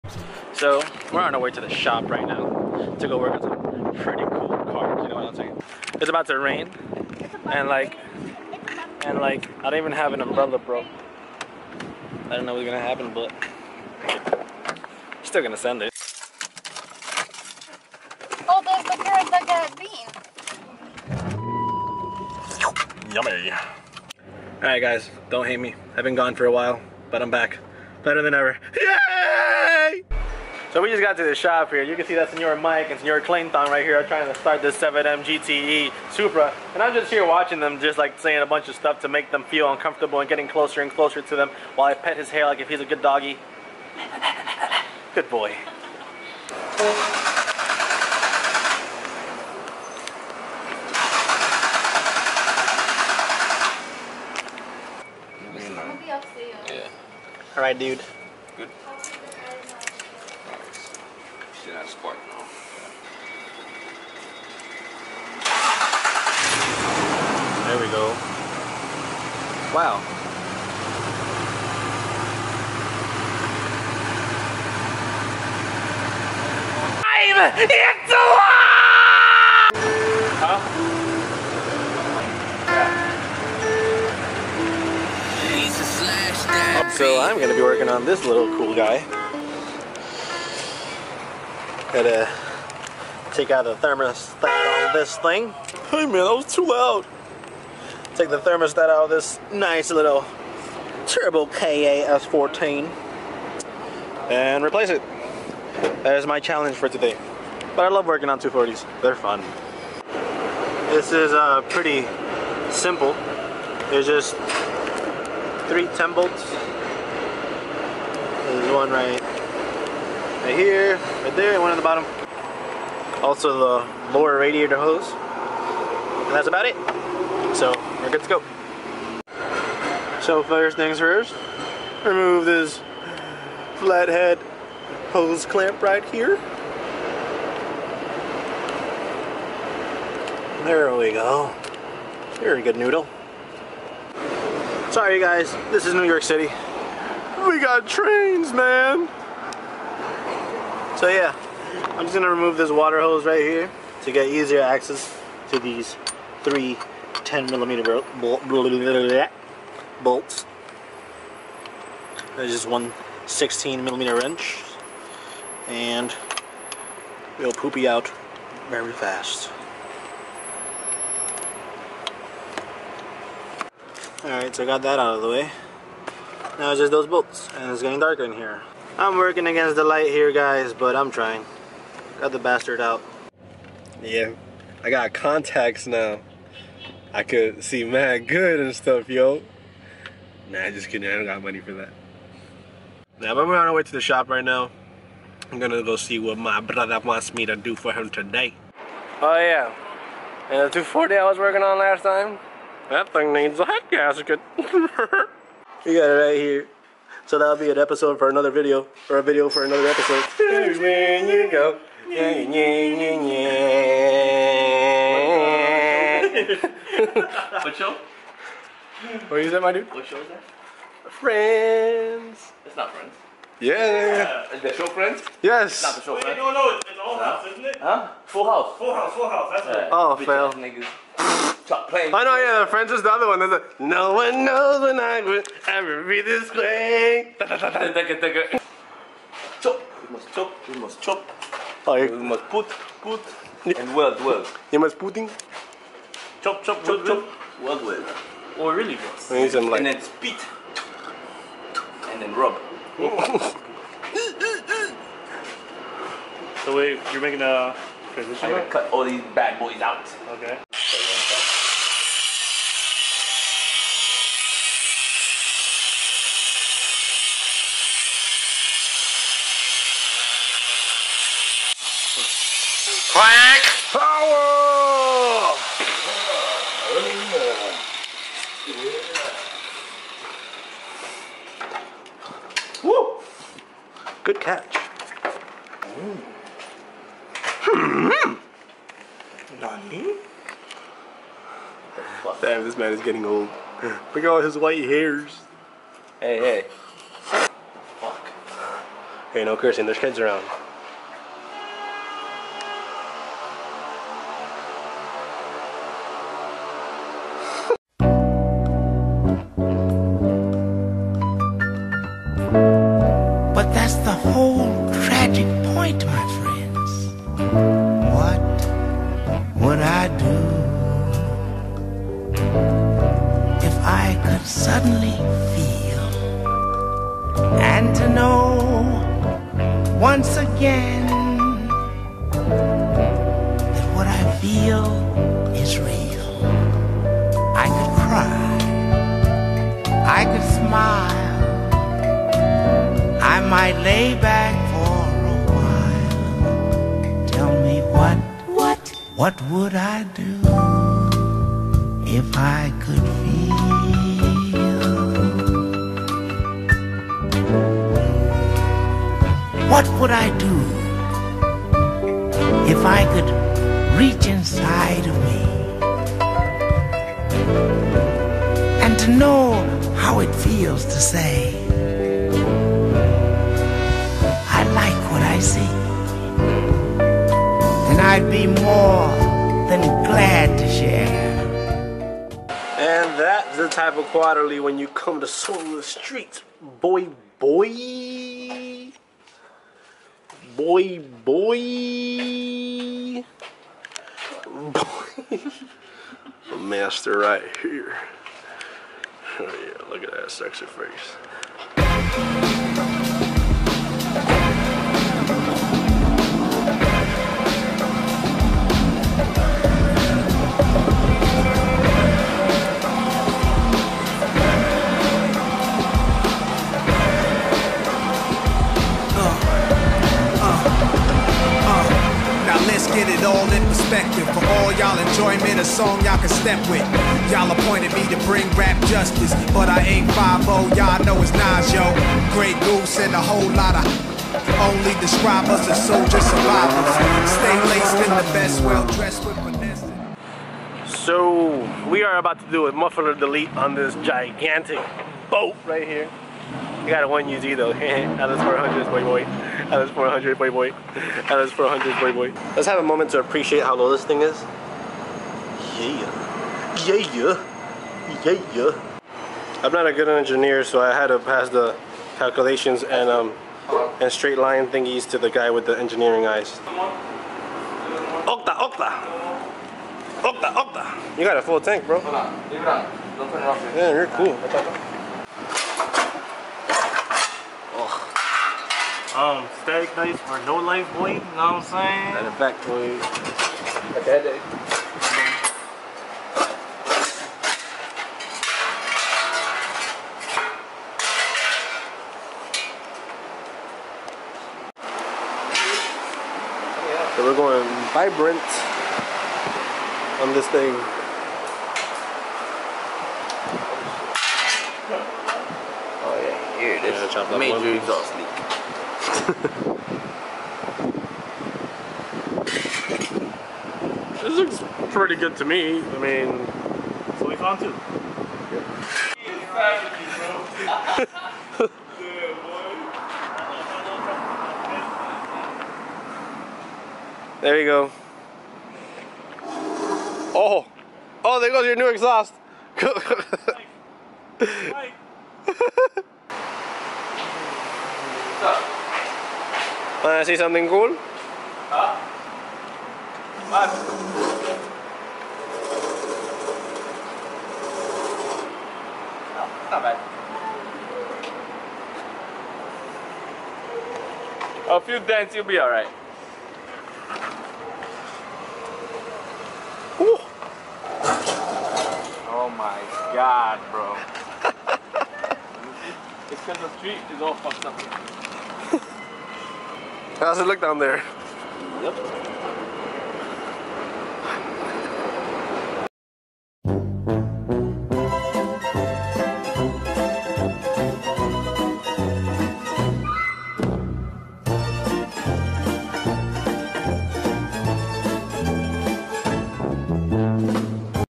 Monday's With Goober: WORKING ON THE 7MGTE SUPRA AND THE S14
On this episode of Monday's With Goober I will be Going to my brothers shop @soul.ofthe.street to give him a hand. I've been gone for a while but im back so you guys will be getting more content!